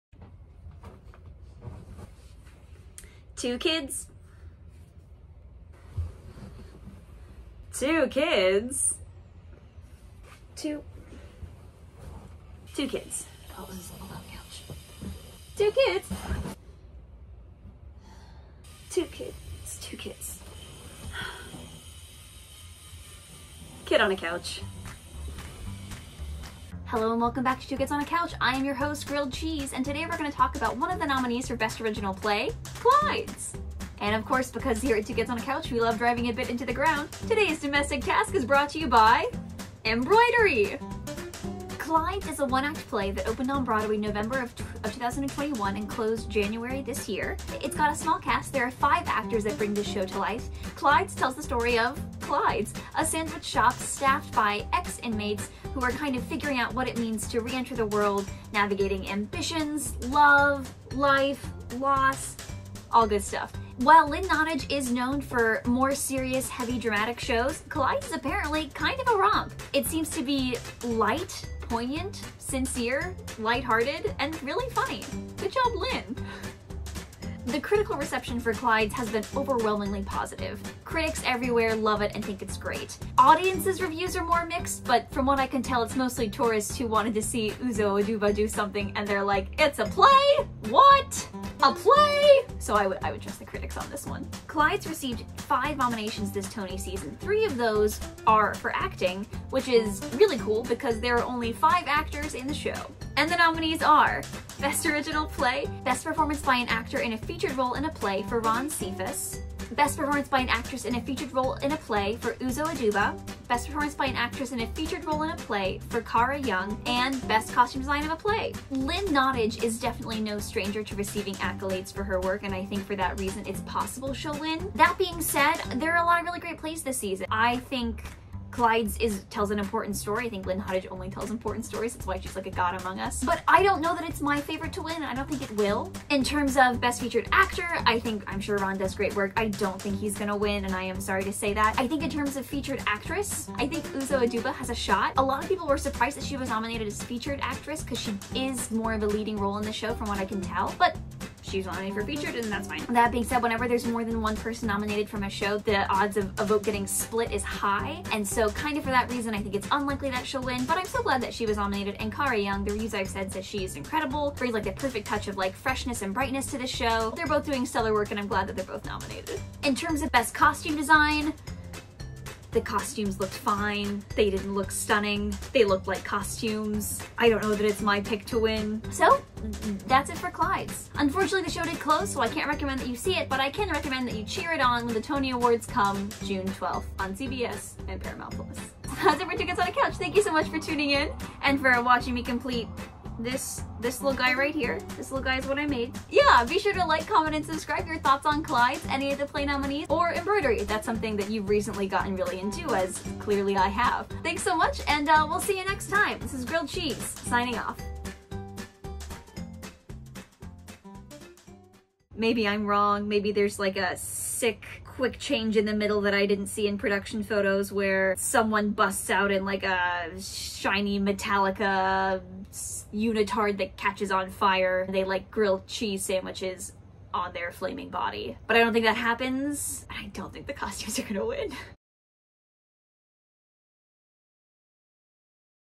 Clyde's
Feeling peckish? Well grab a snack, take a seat, and get ready to learn about Lynn Nottage's newest slice of delicious theatre: Clyde's! From world class sandwiches to the throes of new love, this show has something for everyone. Five nominations later, how do you think this show will fare come June 12th? Validate us!

Follow us on Things!